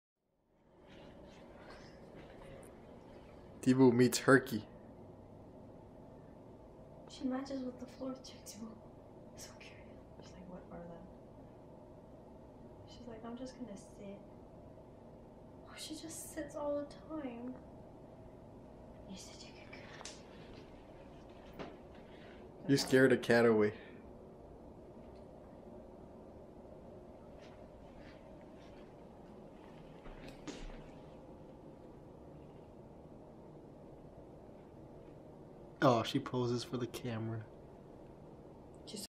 Herky the cute Cavalier King Charles puppy meets Cat
We took Herky to meet Tibou on her first night with us. She scared the cat away.